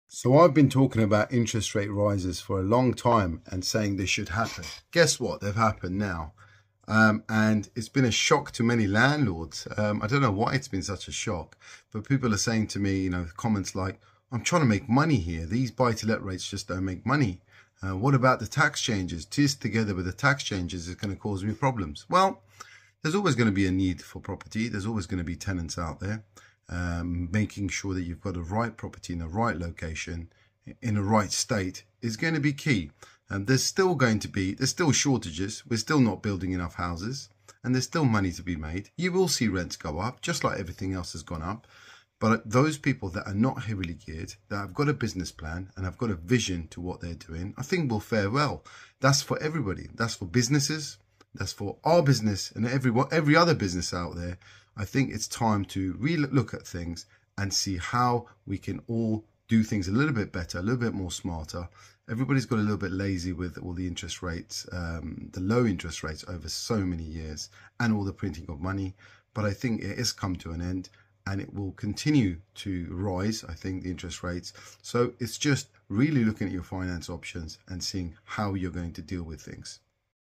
Landlord comments
Some of the conversations on having with the existing landlords about mortgage products and the interest rate Rises